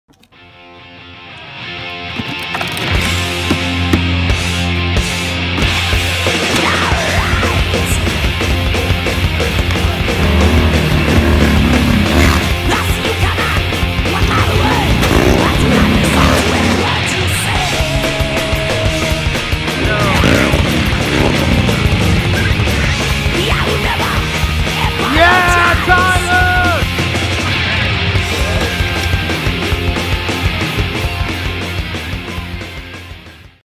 Tyler Flight’s insane Harley jump
He walked away with a concussion.
240p for your viewing pleasure.